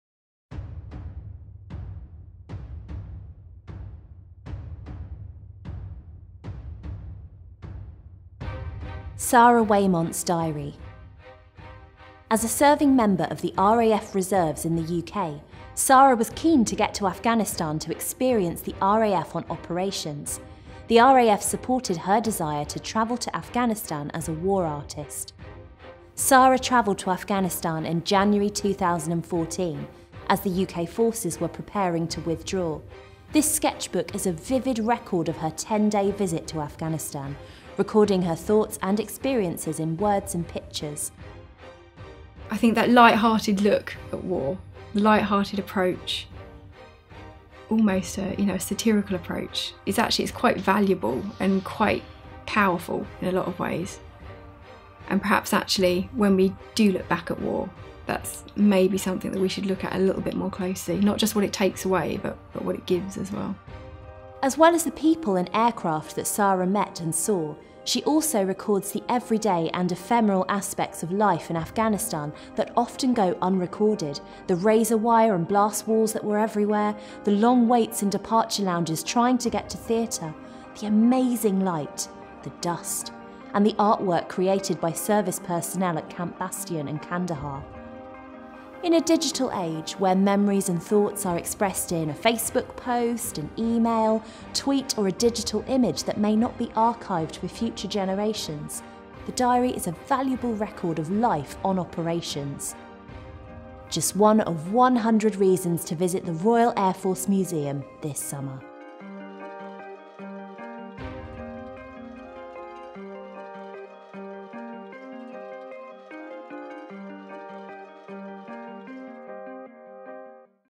Sara Waymont's Diary - One of 100 reasons to visit the RAF Museum this summer
In 2014 RAF Reservist Sara Waymont spent 10 days in Afghanistan as an official war artist for the RAF.

View her diary on display in 'The RAF in an Age of Uncertainty: 1980 - Today', 1 of 3 new permanent exhibitions opening to the public on 30 June as part of our London site's multi-million pound redevelopment.

Just one of 100 reasons to visit the RAF Museum this summer. to discover yours.